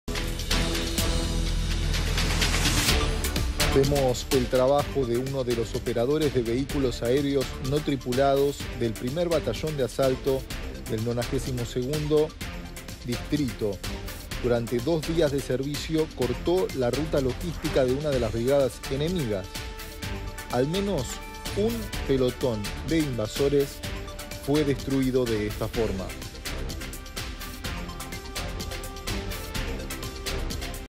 GUERRA RUSIA - UCRANIA | Nuevos combates entre tropas de Putin y Zelenski
👉  El conflicto bélico comenzó el 24 de febrero del año 2022 y el número de víctimas fatales continúa aumentando.